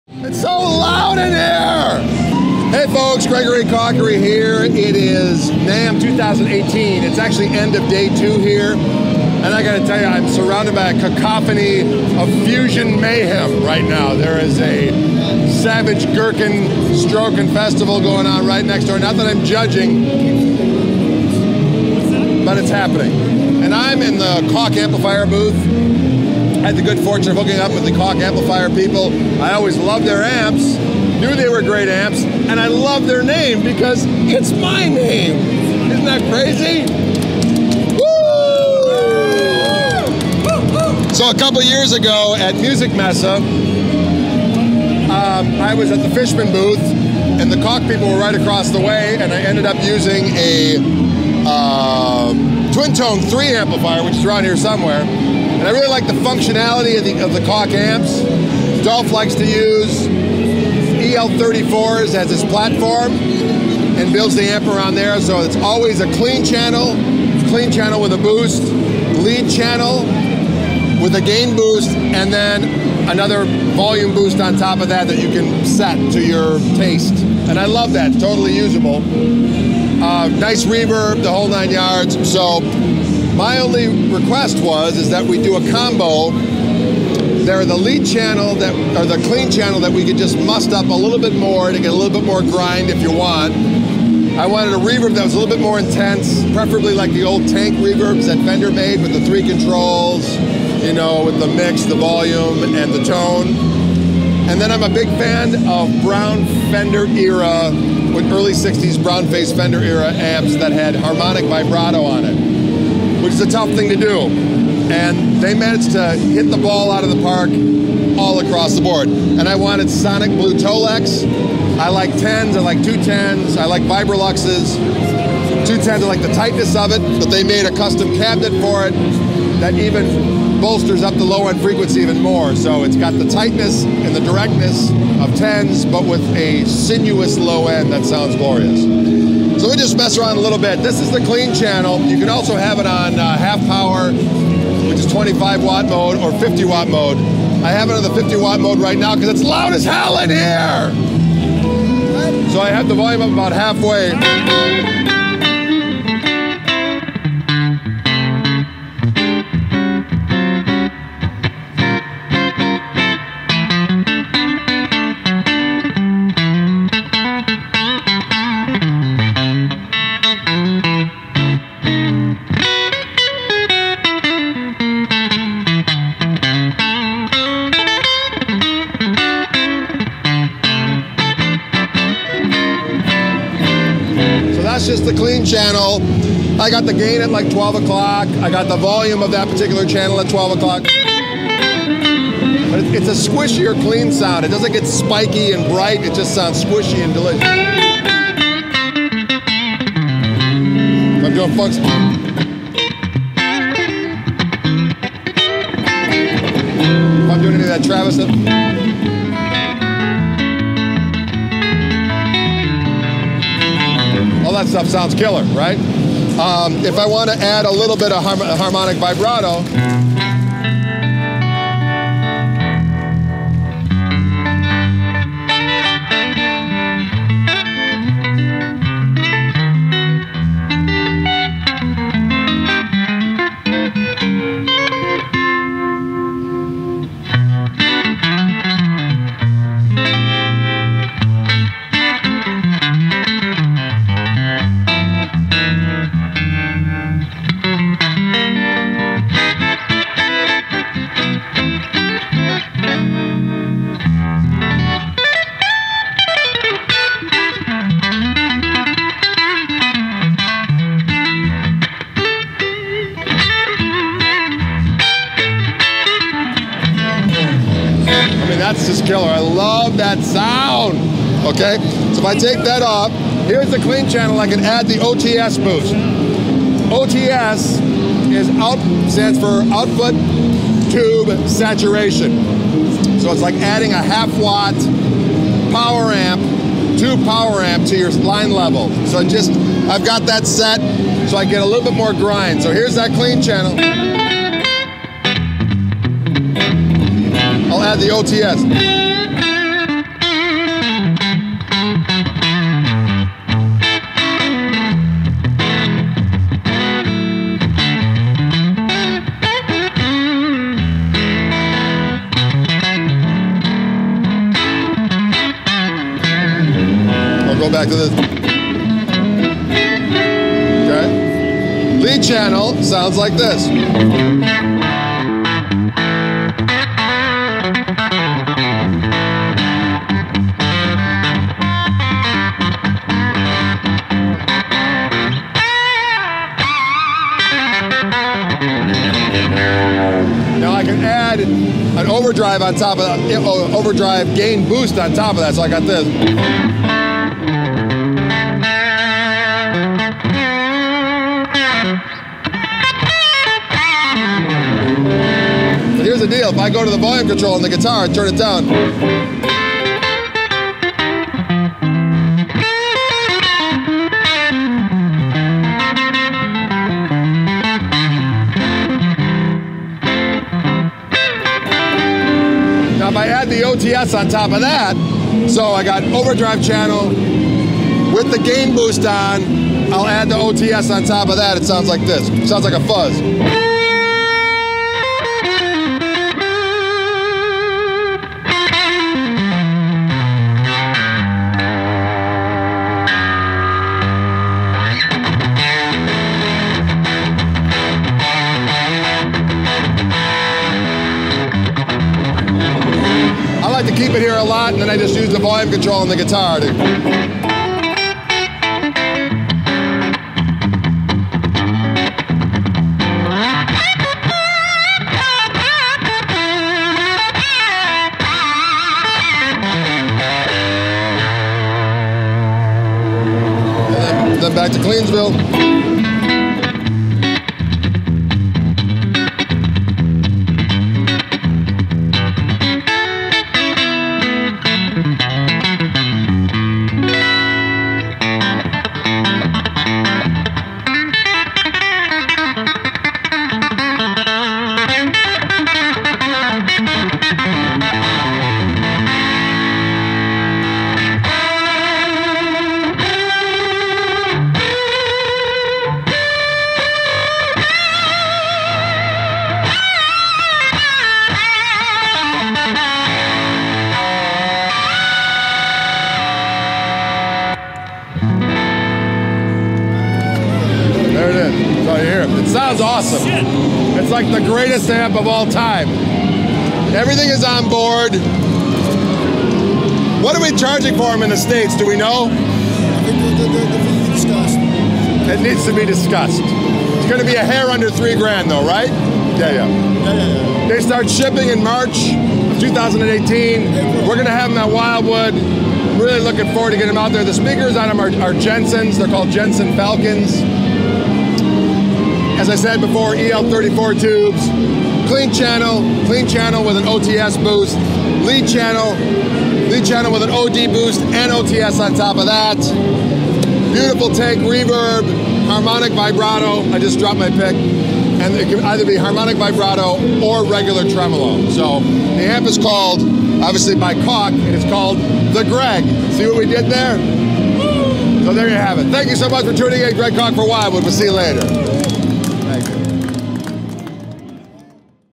Greg Koch's Signature Amp, "The Greg" from Koch Amps • NAMM 2018
It's that time of the year again, ladies and gentlemen!!  We're back in Anaheim for the NAMM Convention roaming the halls for everything new and exciting.  And boy, this year did not disappoint!  Each of our wonderful manufactures provided us with in-depth and exciting tours of their new offerings.

In this video we get a special tour of Greg Koch's own signature amplifier from Koch Amps!!  Meet "The Greg"!